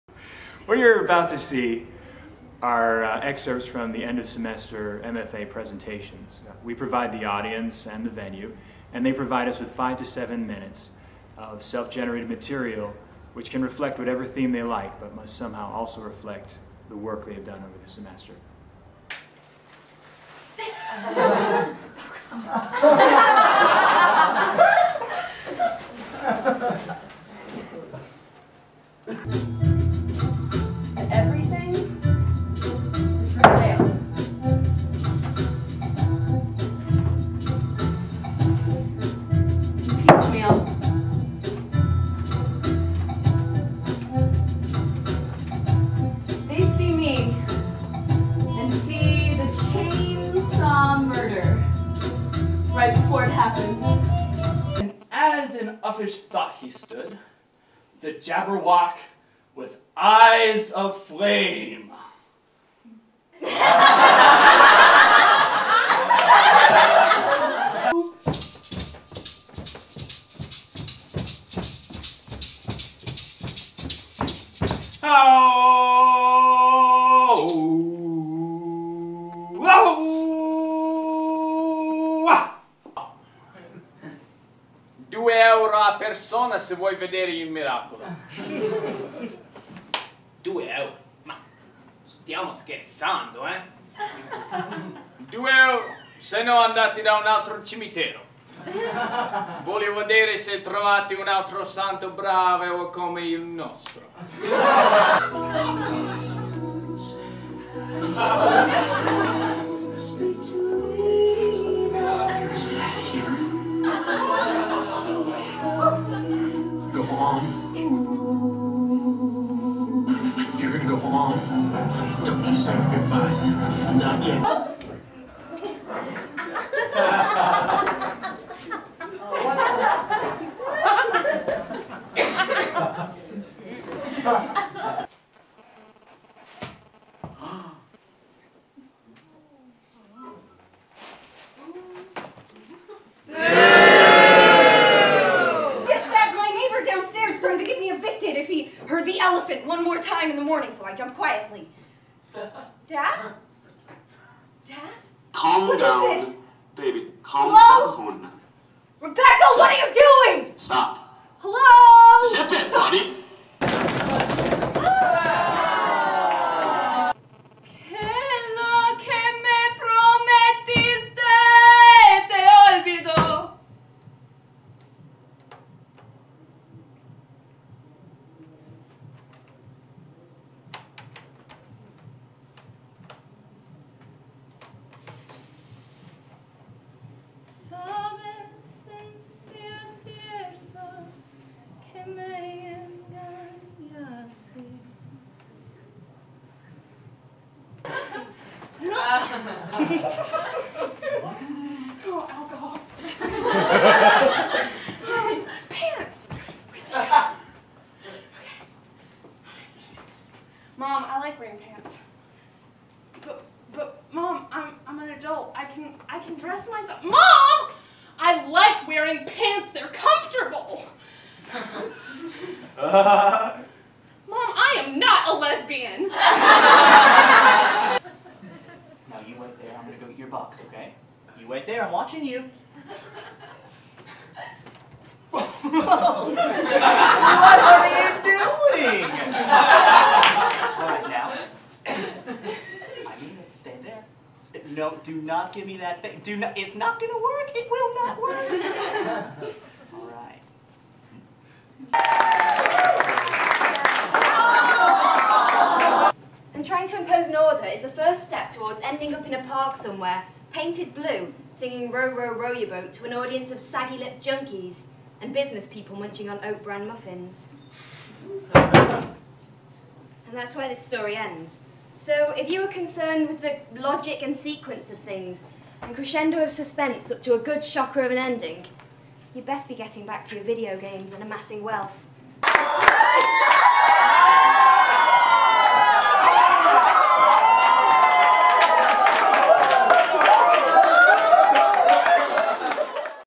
Accademia dell'arte MFA Solo Projects
MFA solo project clips that were performed in Arezzo, Italy at the Aurora.